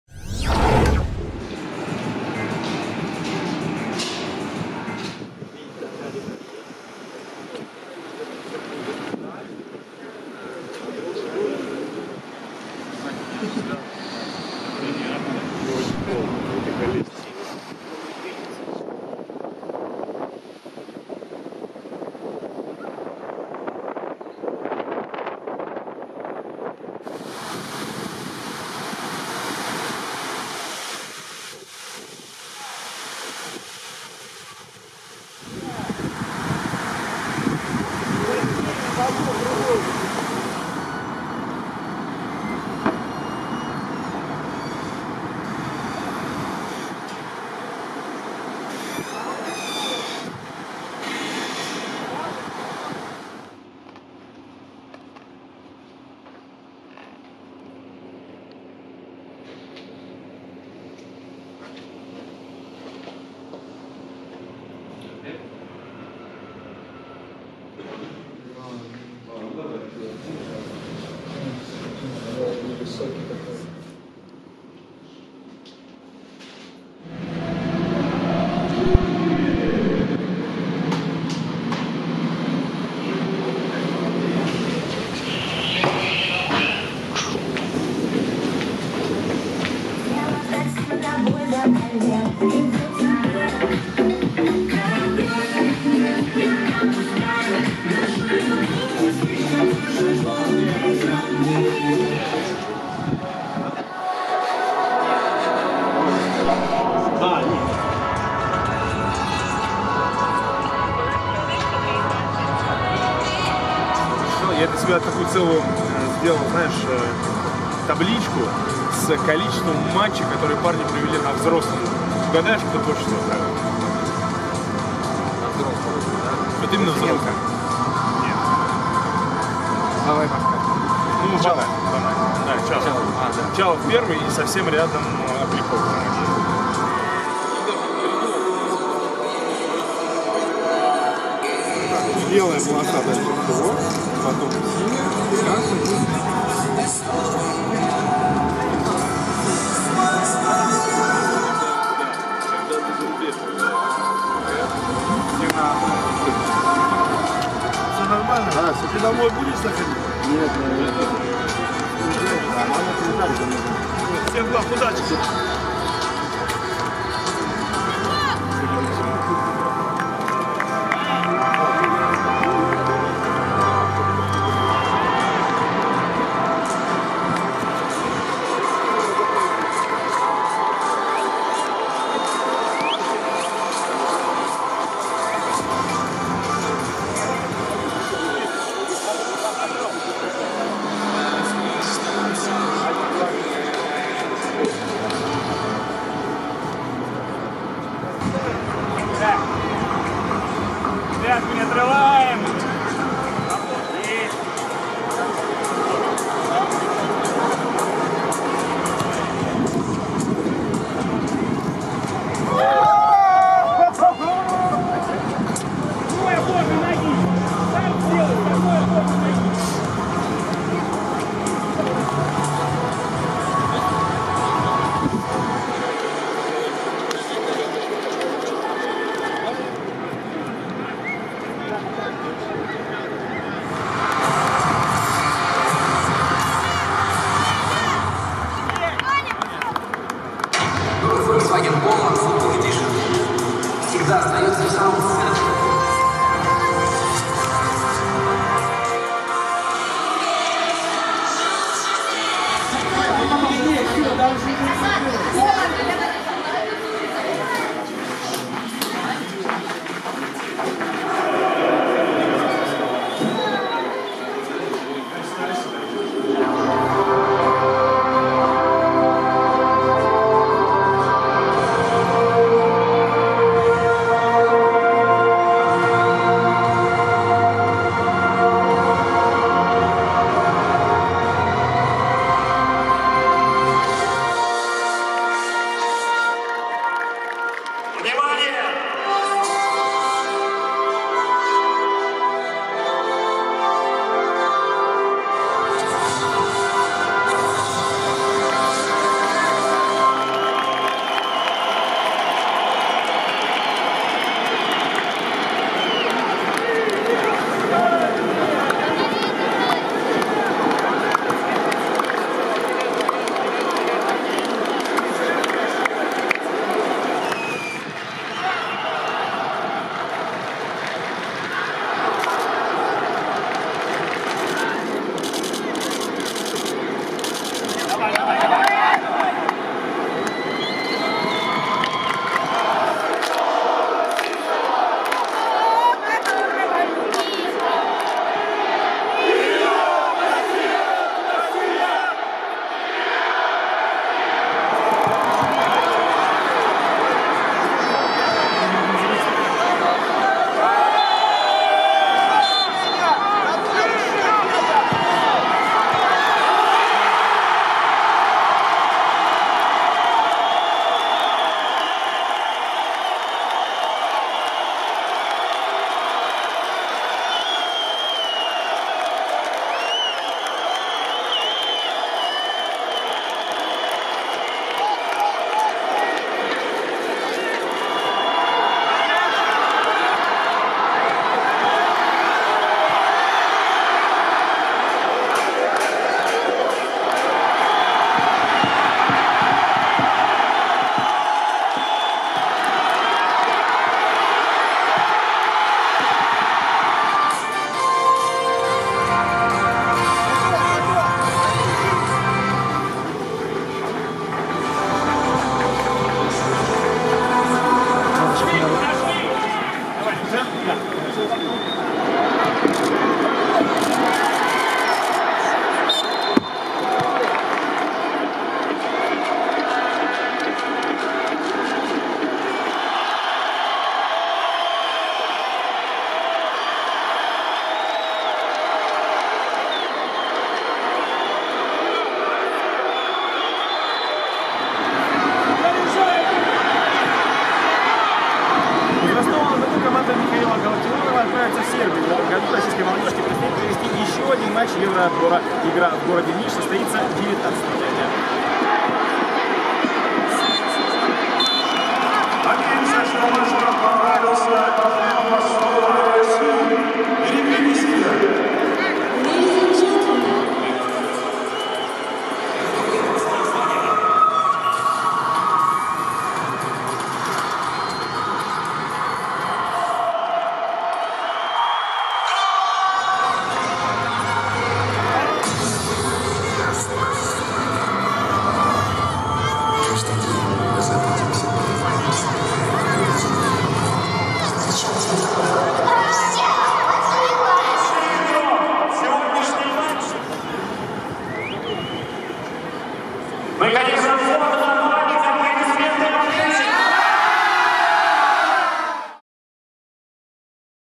Молодежка. Россия — Латвия: за кадром | РФС ТВ
Молодежная сборная России в пятом туре отборочного турнира чемпионата Европы 2021 года в Ростове-на-Дону обыграла со счетом 2:0 сборную Латвии и благодаря успеху сборной Болгарии в матче против сборной Польшой возглавила свою группу. На данный момент на счету подопечных Михаила Галактионова 11 очков. Традиционная рубрика "За кадром" расскажет обо всем интересном, что не попало в объективы трансляции матча.